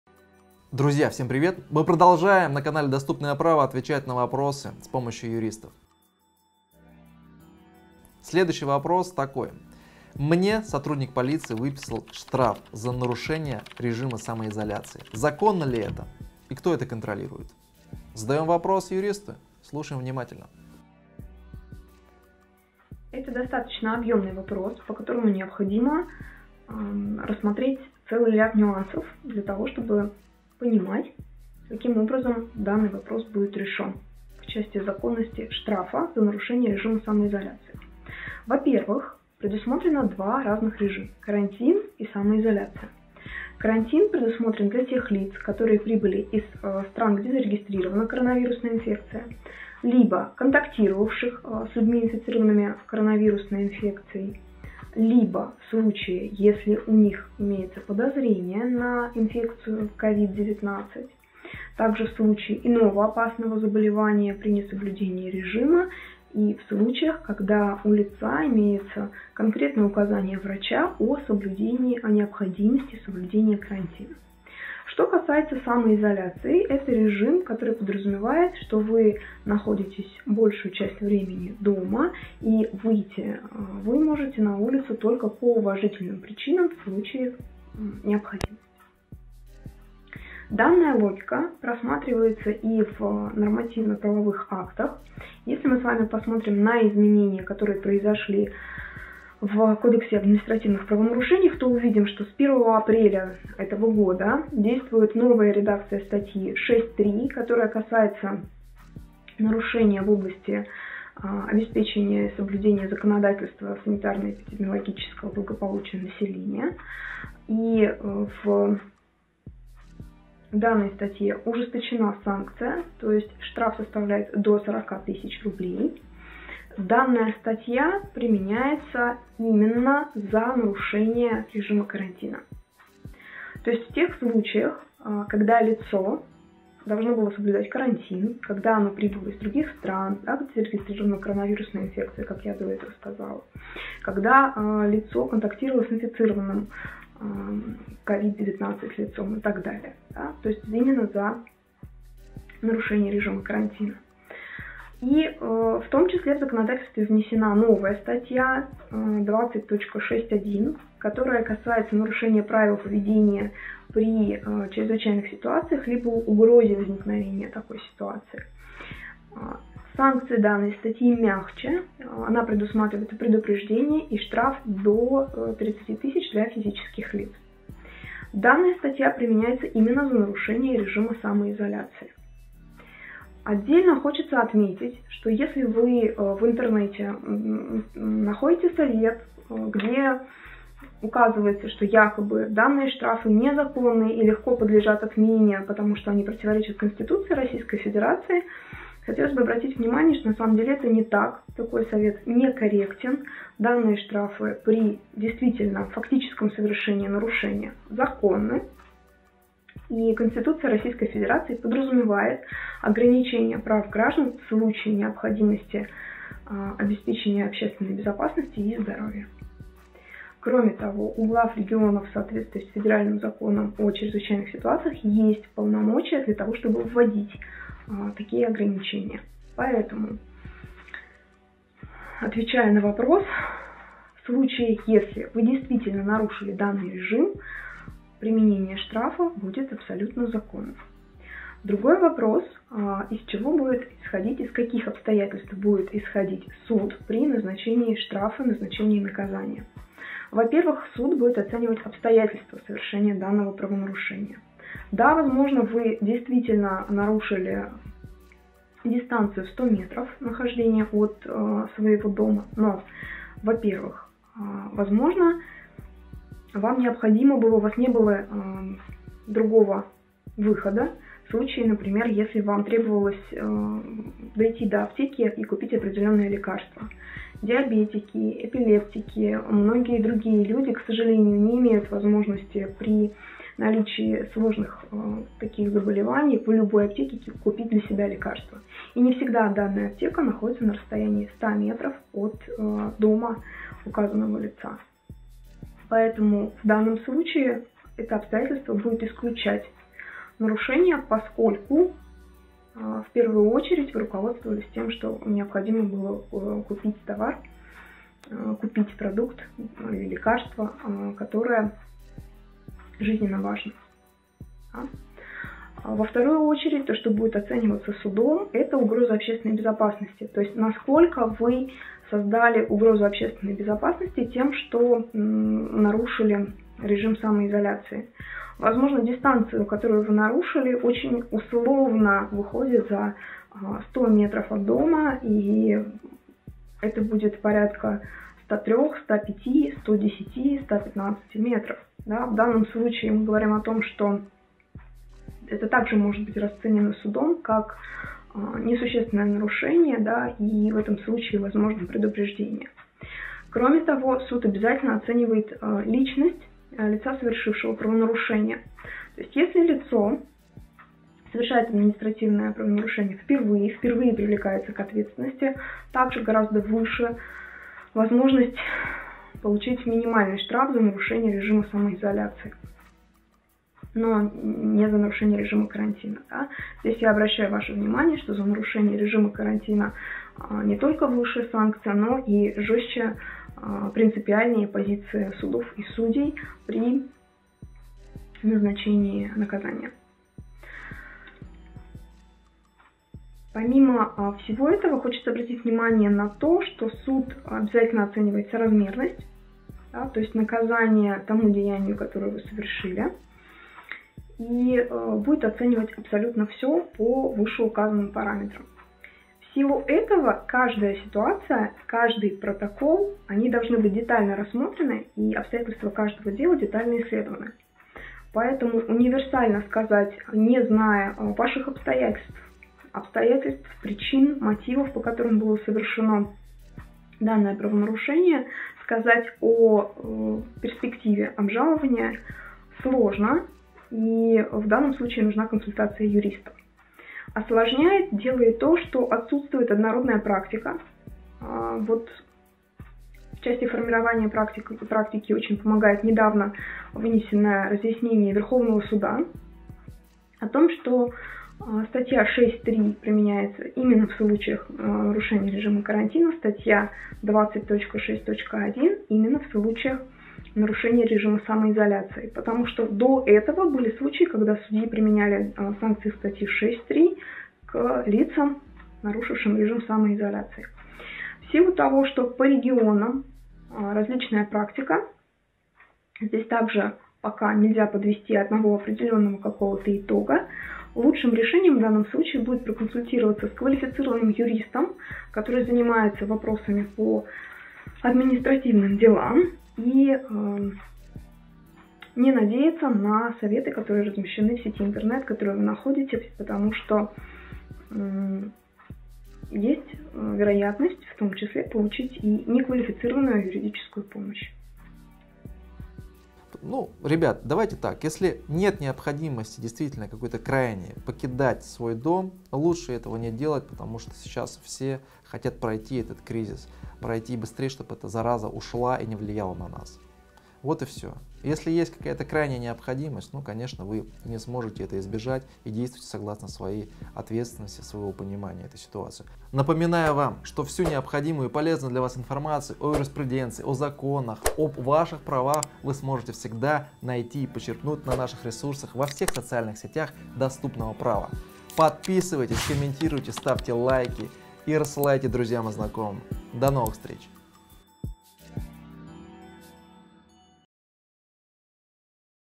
ШТРАФ ЗА НАРУШЕНИЕ САМОИЗОЛЯЦИИ
"Сотрудники полиции выписали штраф за нарушение режима самоизоляции. Законно ли это?"

Новое видео на канале "Доступное Право"

чем отличаются меры наказания за нарушение самоизоляции и за нарушение карантина.
Мы сотрудничаем с опытными юристами. Если вам нужна консультация, переходите по ссылке и оставляйте заявку на сайте😉